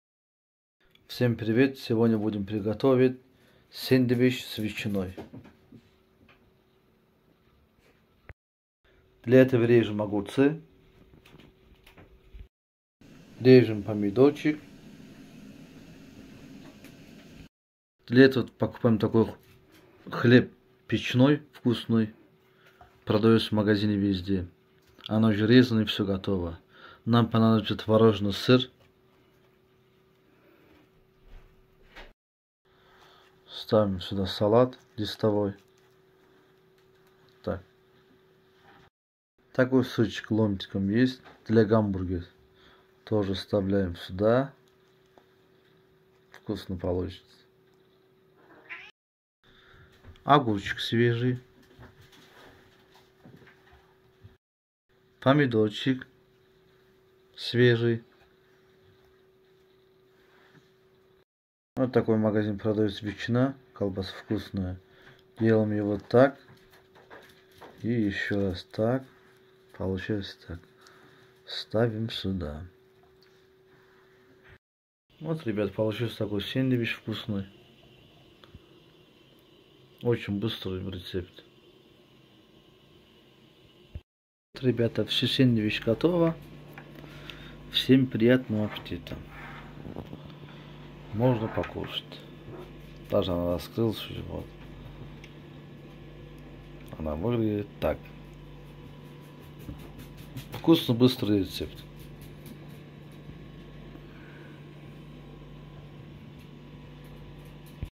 Сэндвич с ветчиной и сыром 🥪🥪🥪🧀🧀🧀🤤🤤🤤😋😋😋👌👌👌
салат листовой
колбаса ветчина
помидор свежий
огурцы свежие
сыр творожный
хлеб пшеничный
сыр для гамбургер